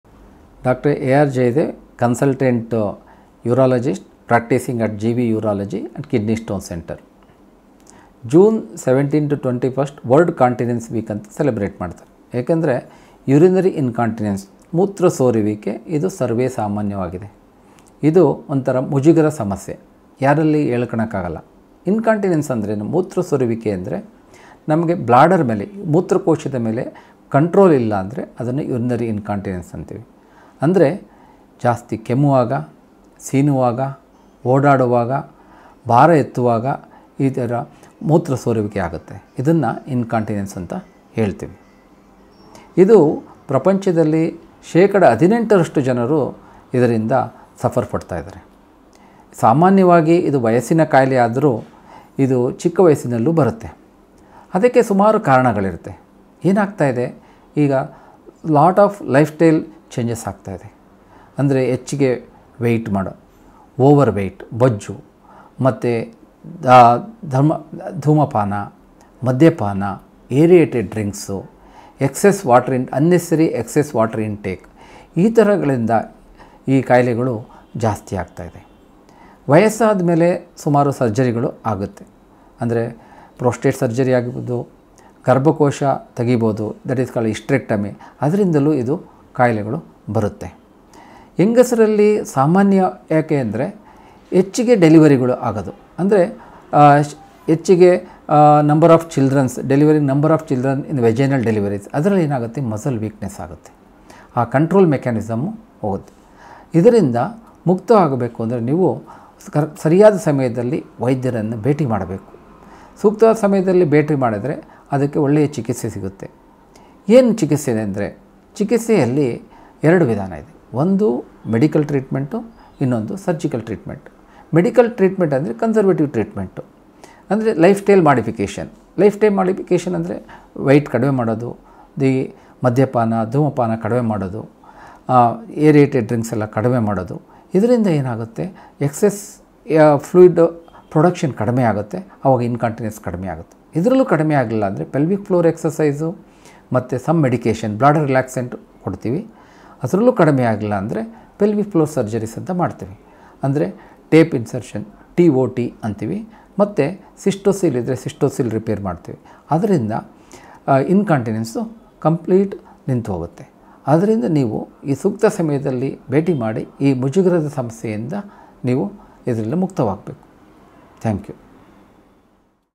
Corporate Video - Dr A R Jayadev
Dr A R Jayadev Spreading the Awareness of World Continence Week